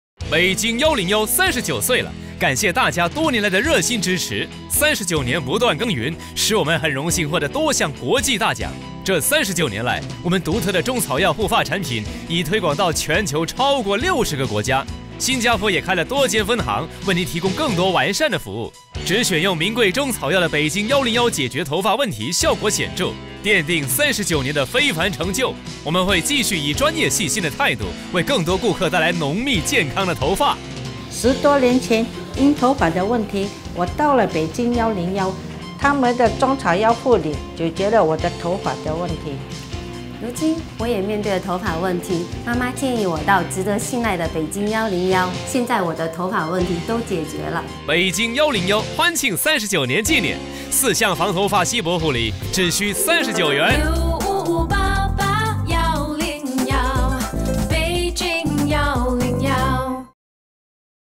39th Anniversary Event Ad
Come celebrate our 39th Anniversary with us.
Enjoy our promotion - 4 Anti Hair Thinning Treatments @ $39